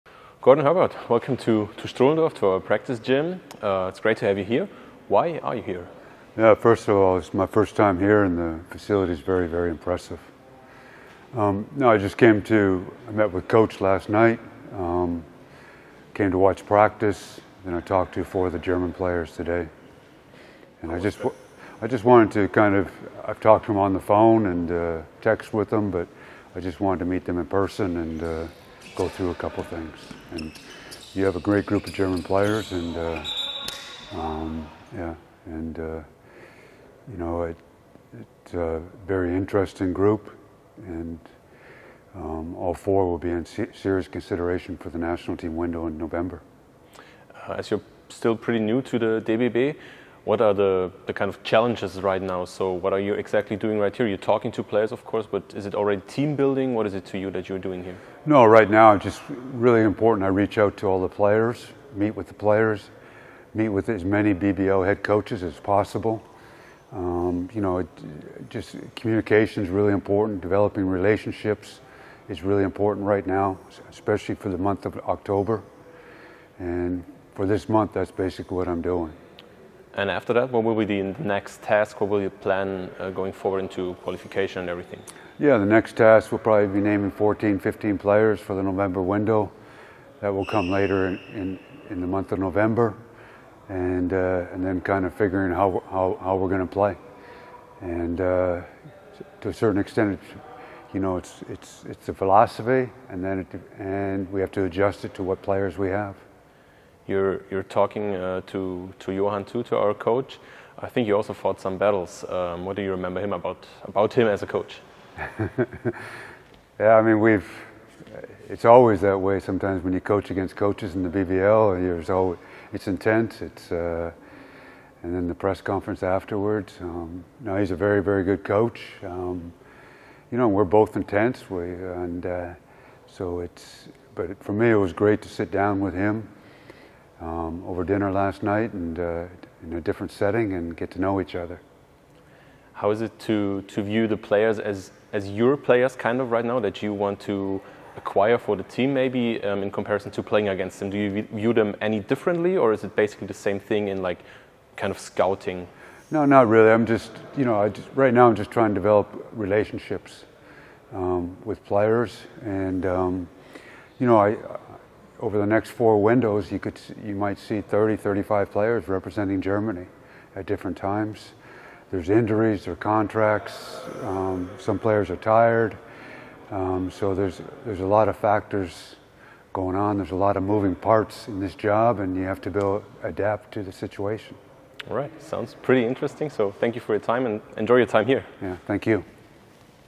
„Ich versuche Beziehungen aufzubauen!“ 💬 | Gordon Herbert zu Gast bei Brose Bamberg 👋
Prominenter DBB-Besuch in unserem Trainingszentrum! 👀 Bundestrainer 🇩🇪 Gordon Herbert war heute zu Gast! 👋 Mit welchen Spielern er gesprochen hat, verrät er zwar nicht, dafür erklärt er, was momentan so ansteht! 🎤

Alle Infos rund um Brose Bamberg gibt's unter brosebamberg.de und auf den Social Media Kanälen: